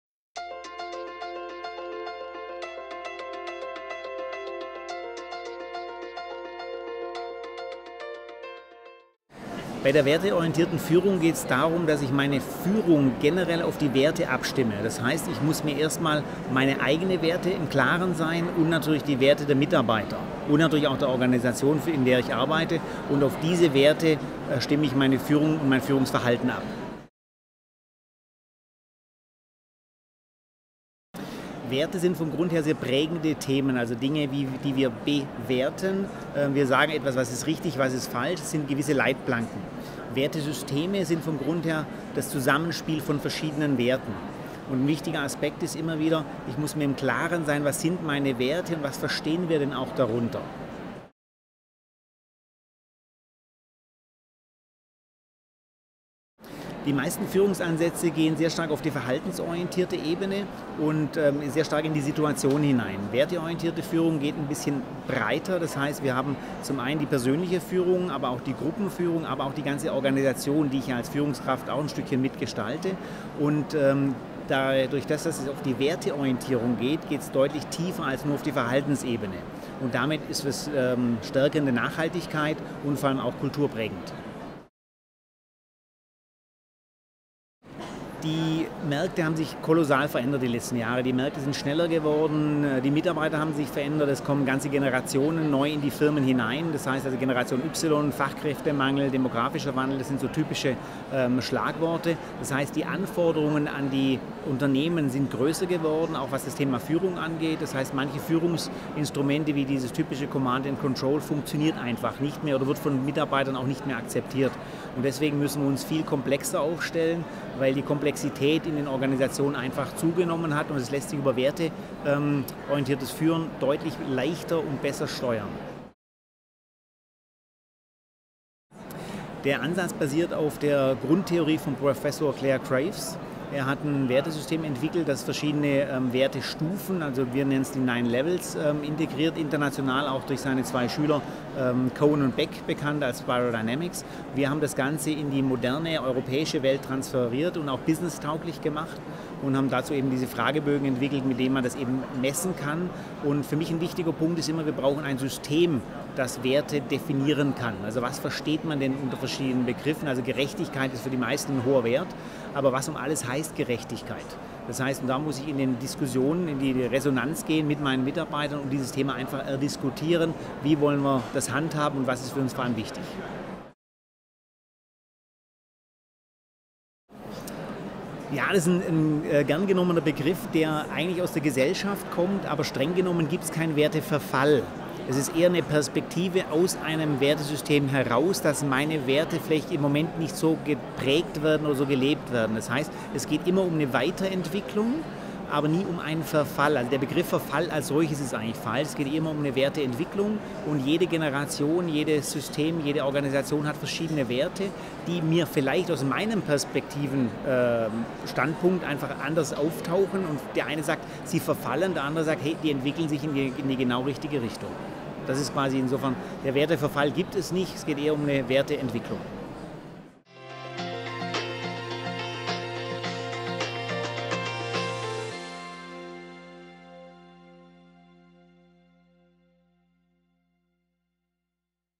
Werteorientierte Führung - Interview mit Rainer Krumm
Führung gelingt, wenn die Werte der Führungskraft gut zusammenspielen mit den Werten der Organisation und denen der Mitarbeiter. Rainer Krumm setzt auf die Erkenntnisse von Clare W. Graves.